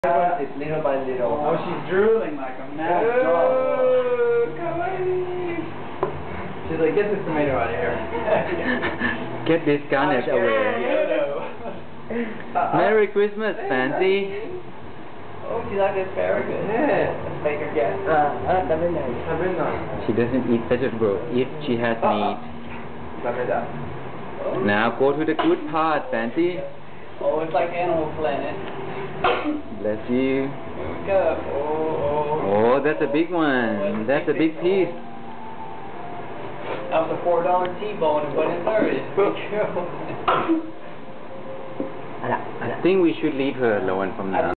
Bulldog Fancy and her Argentina beef
Facny enjoying a t-bone from Buenos Aires